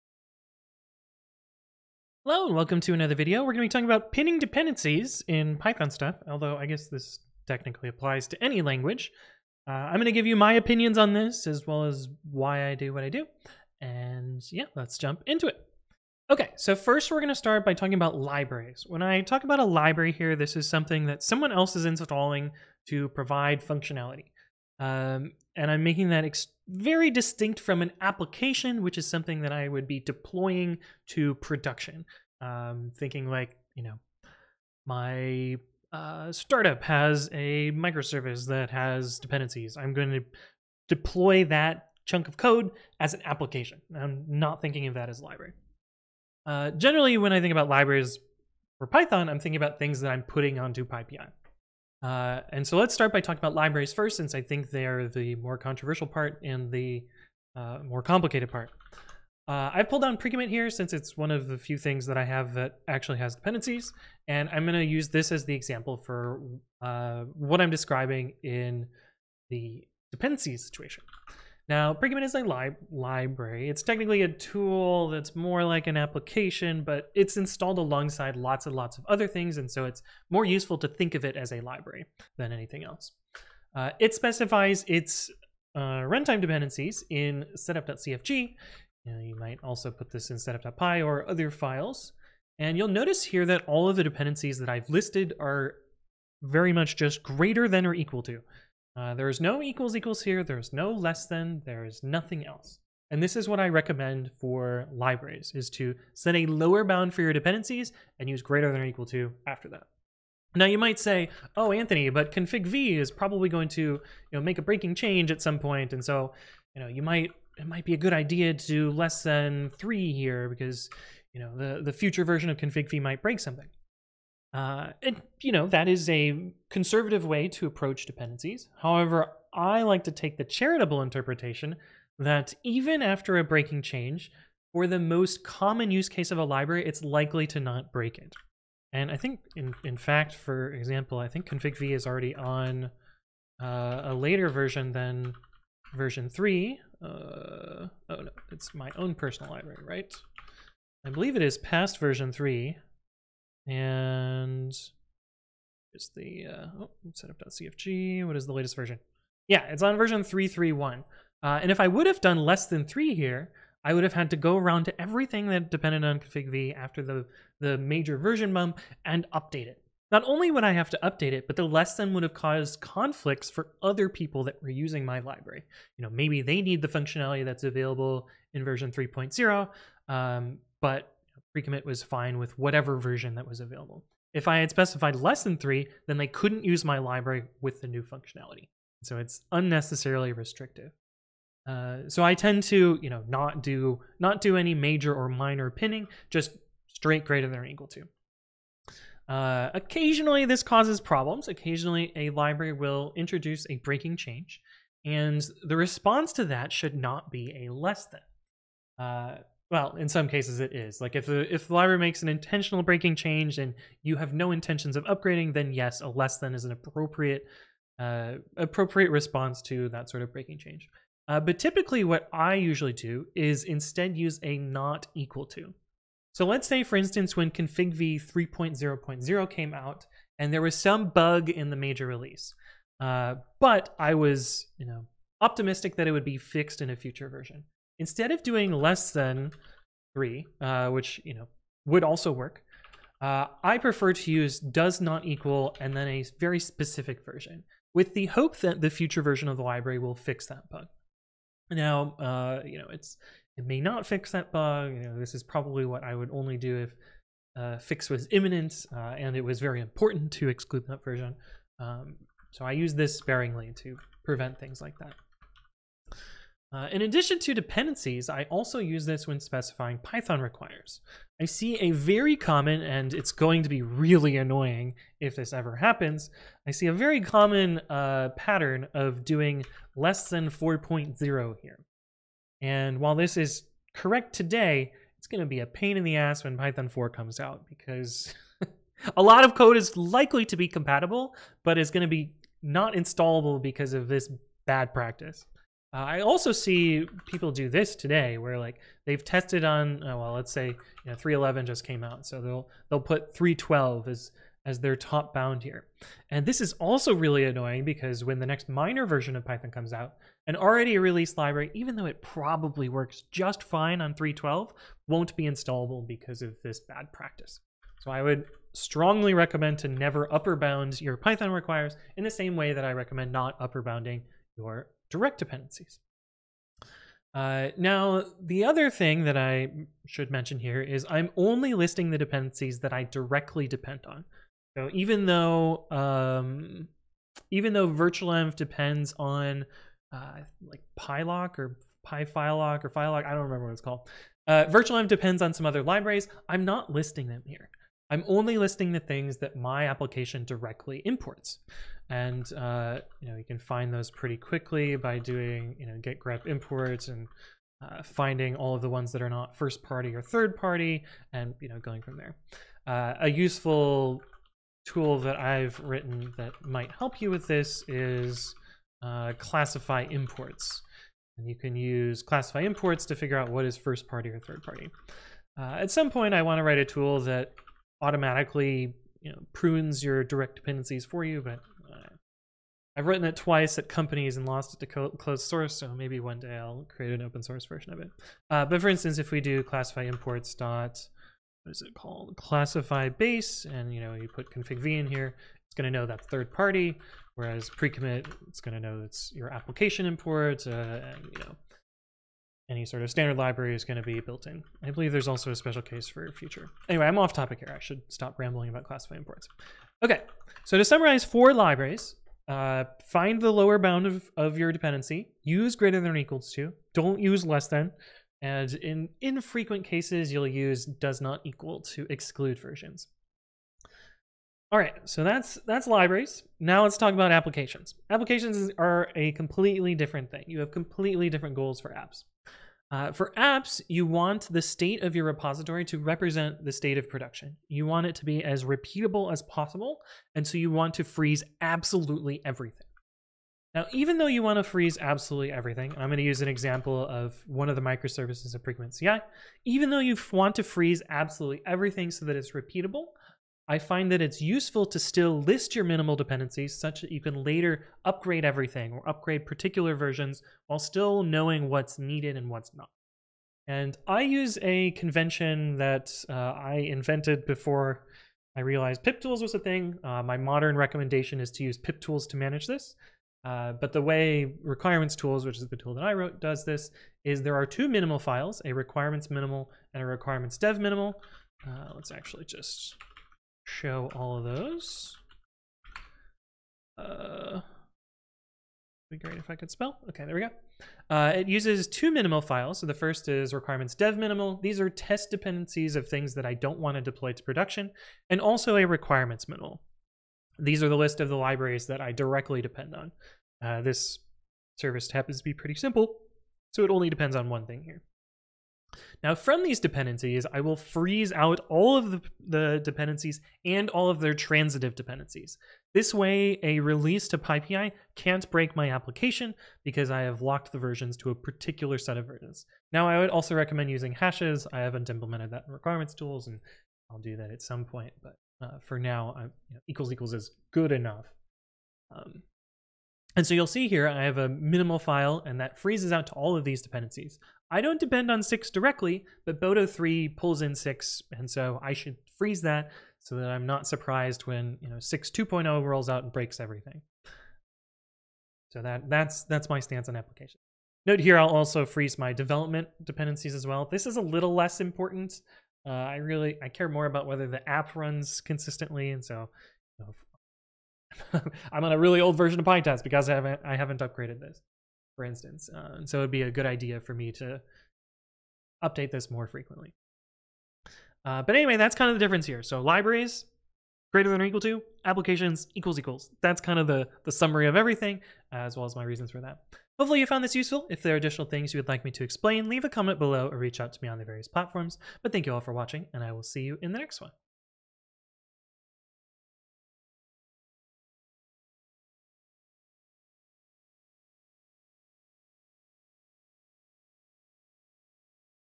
when should I pin deps: never and always! (intermediate) anthony explains #514
fine I'll make a video about it -- here's my stance on pinning dependencies and why I do what I do in the different scenarios.